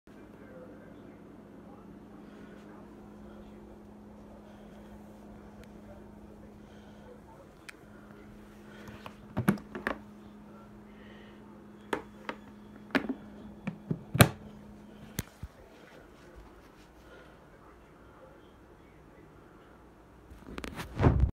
2020 Coleman CT200U EX R
Just bought it saved up some money and I paid $550 from rural king